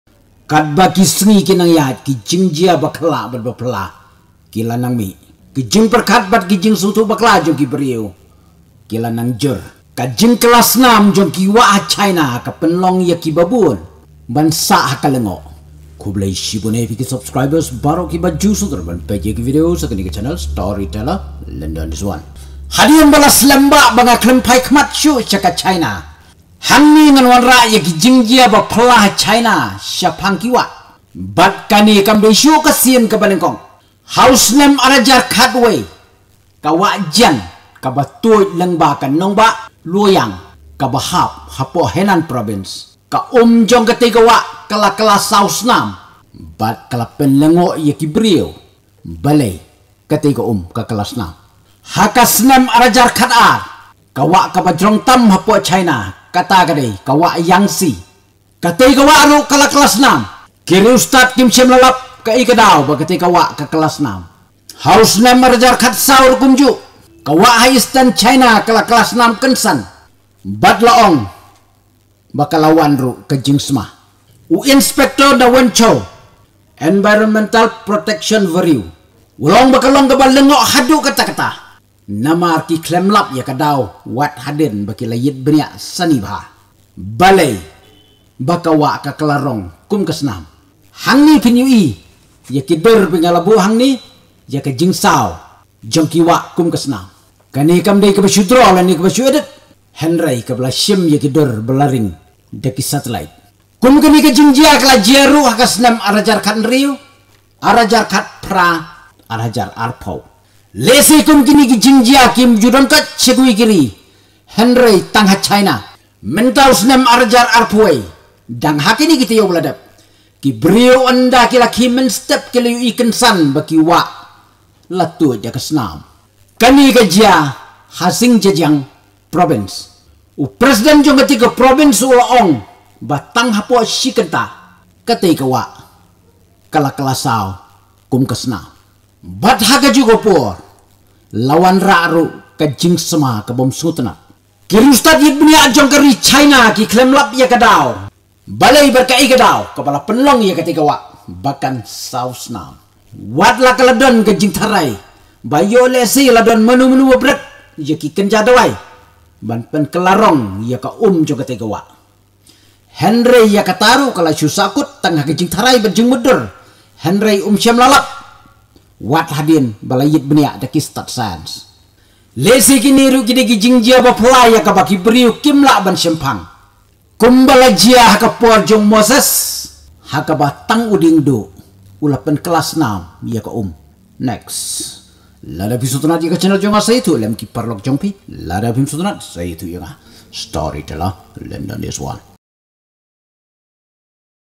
kylla snam ki wah ha china//education video
history about the bloody river
storyteller lyndon is one